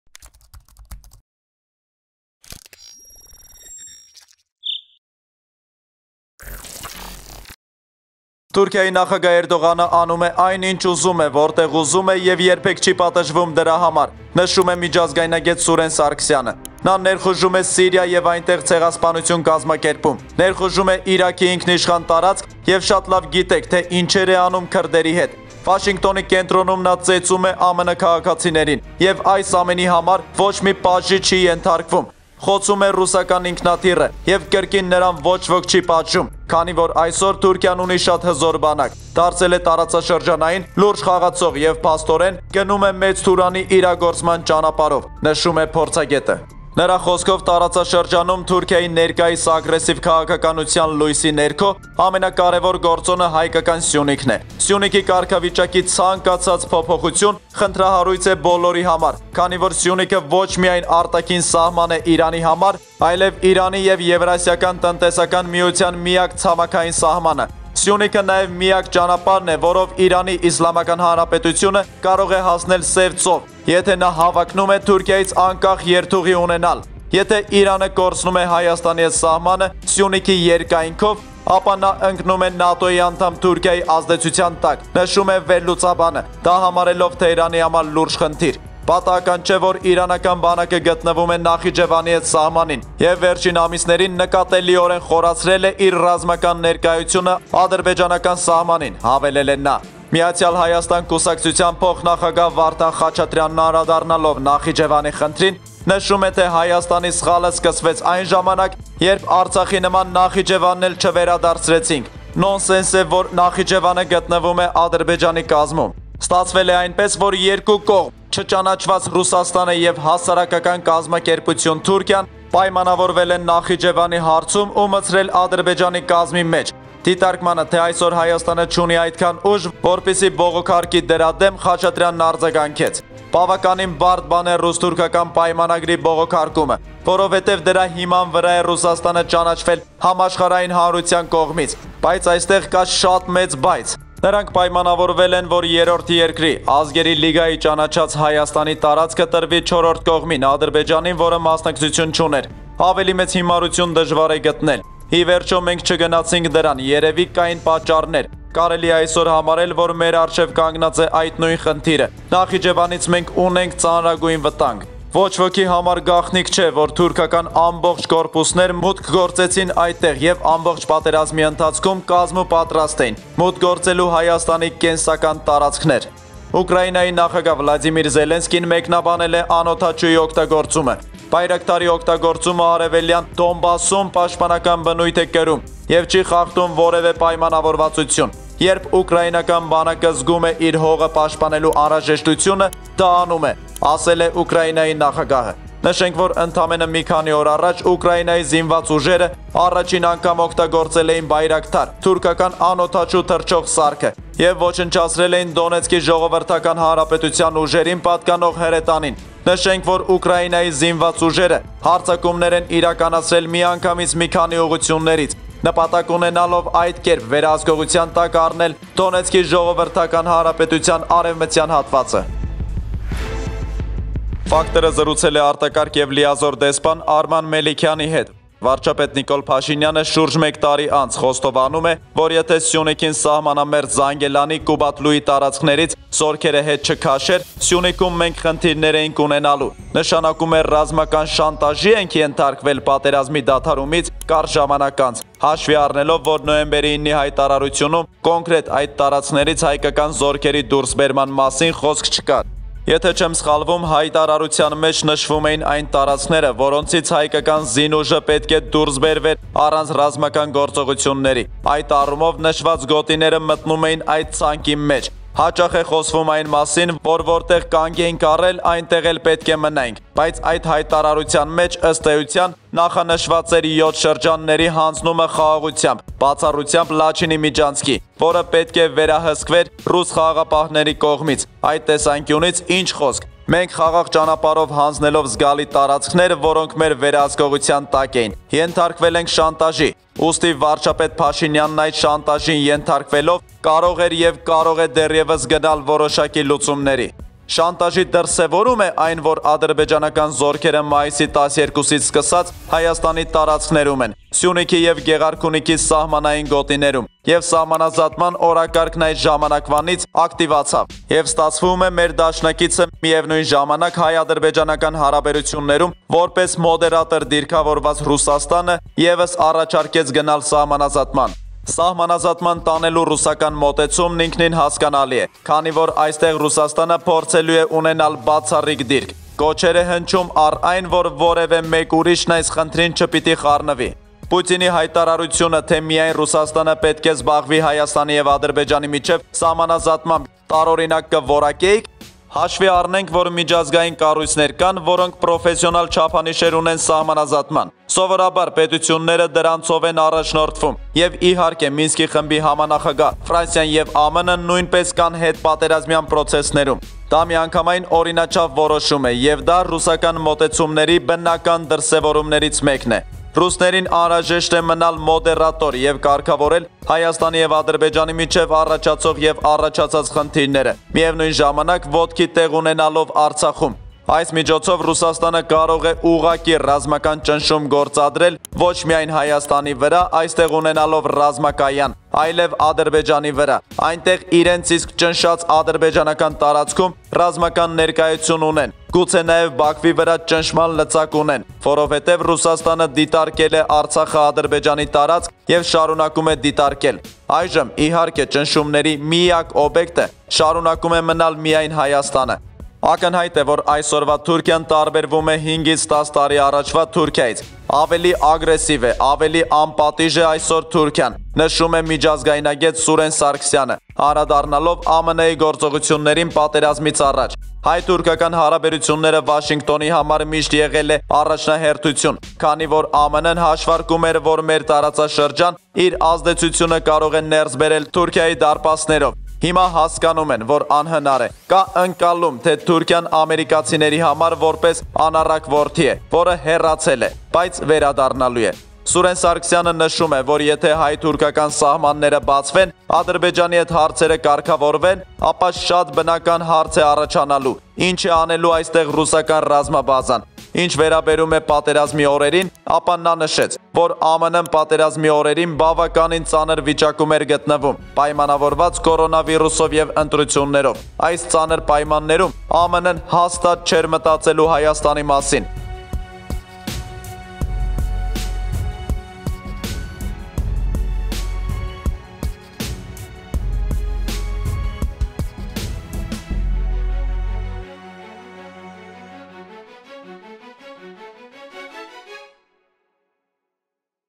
ԱՄՆ հասկացավ, որ անհնար է․ Նախիջևանի վտանգը․ Իրանն այլ տարբերակ չունի․ Զելենսկին խոստովանեց
«Թուրքիայի նախագահ Էրդողանը անում է այն, ինչ ուզում է, որտեղ ուզում է, և երբեք չի պատժվnւմ դրա համար», - նշում է միջազգայնագետ Սուրեն Սարգսյանը:
«Նա ներխnւժում է Սիրիա և այնտեղ ցեղասպանություն կազմակերպում, ներխուժում է Իրաքի ինքնիշխան տարածք և շատ լավ գիտեք, թե ինչեր է անում քրդերի հետ, Վաշինգտոնի կենտրոնում նա ծեծnւմ է ԱՄՆ քաղաքացիներին: Եվ այս ամենի համար ոչ մի պատժի չի ենթարկվում։ Խnցnւմ է ռուսական ինքնաթիռը և կրկին նրան ոչ ոք չի պատժnւմ։ Քանի որ այսօր Թուրքիան ունի շատ հզոր բանակ, դարձել է տարածաշրջանային լուրջ խաղացող և փաստորեն գնում է Մեծ Թուրանի իրագործման ճանապարհով», - նշում է փորձագետը։
Նրա խոսքով, տարածաշրջանում Թուրքիայի ներկայիս ագրեuիվ քաղաքականության լույսի ներքո «ամենակարևոր գործոնը» հայկական Սյունիքն է:
«Սյունիքի կարգավիճակի ցանկացած փոփոխություն խնդրահարnւյց է բոլորի համար, քանի որ Սյունիքը ոչ միայն արտաքին սահման է Իրանի համար, այլև Իրանի և Եվրասիական տնտեսական միության միակ ցամաքային սահմանը։
Նյութի աղբյուր՝ 1in.am, factor.am